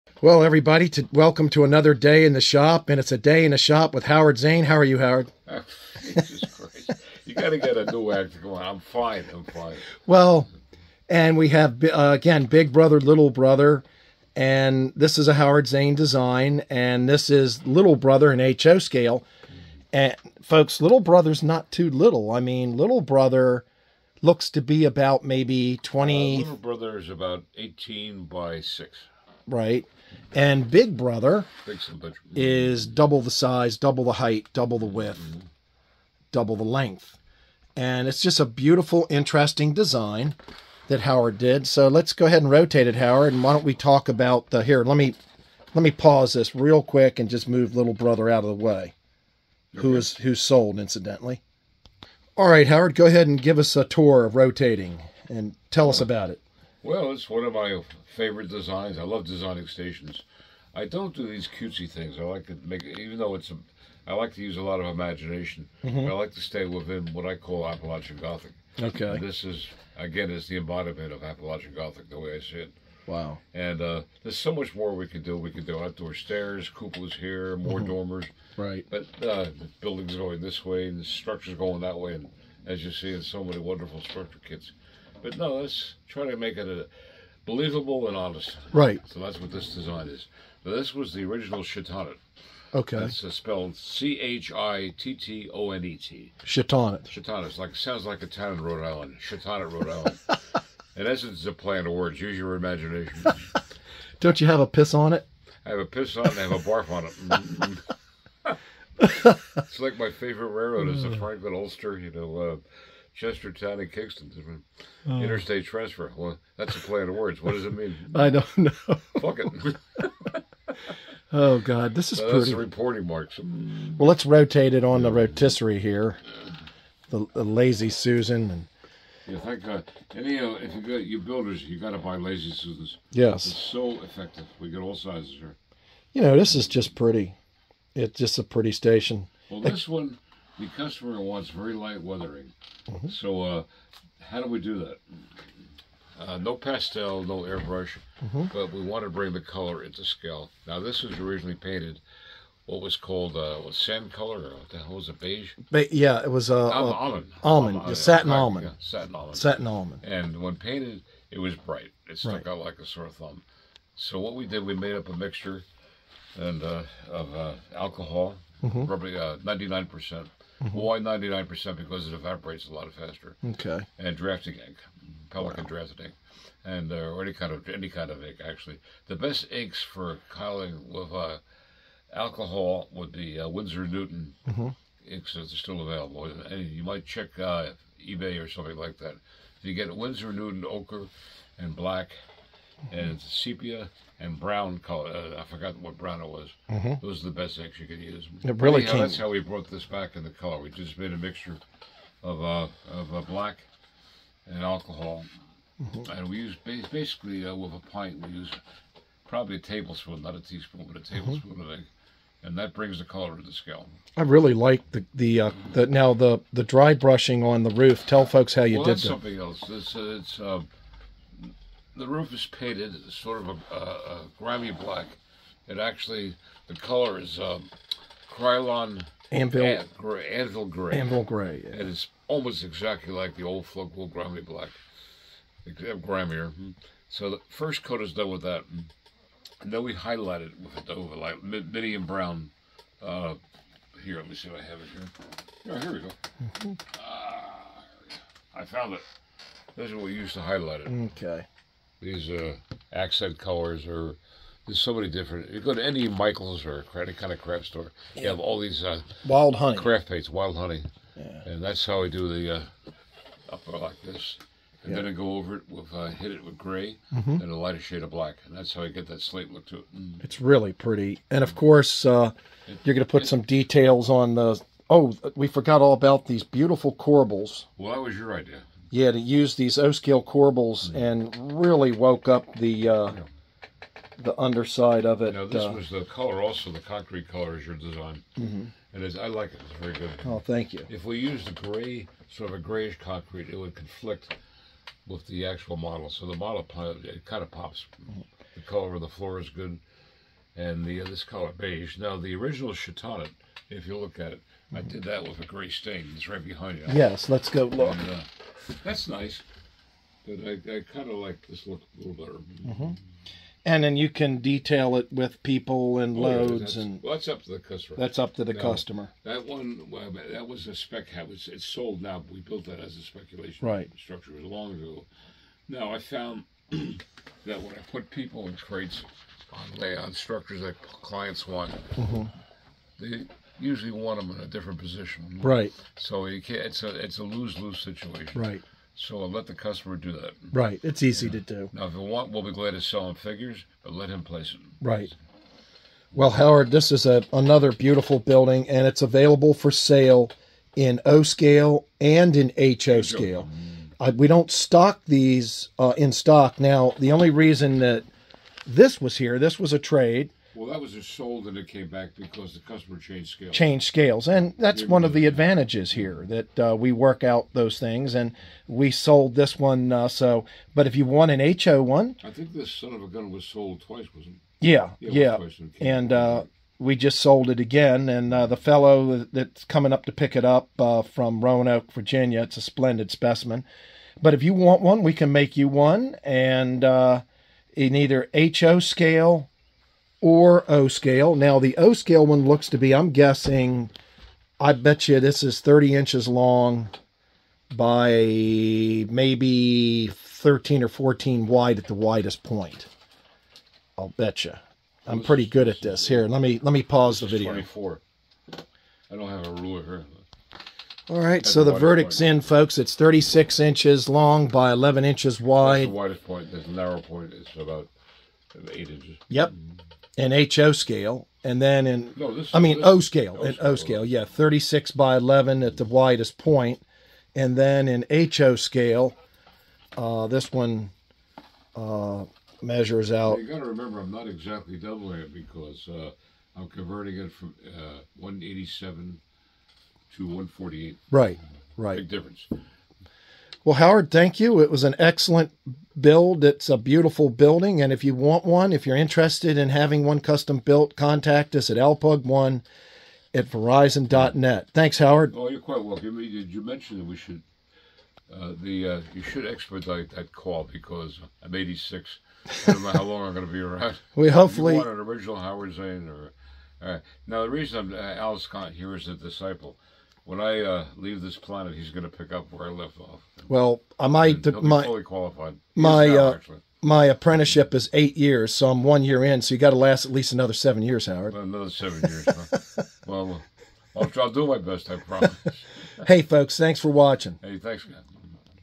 A Day In The Shop With Howard Zane Some New Custom Builds HO and O scales
Here is a Howard Zane design Passenger /Freight station shown in both HO and O scale for comparison. The O scale one is for a High Rail Client and the HO unit is sold but they are custom built to order since all builds are pre sold.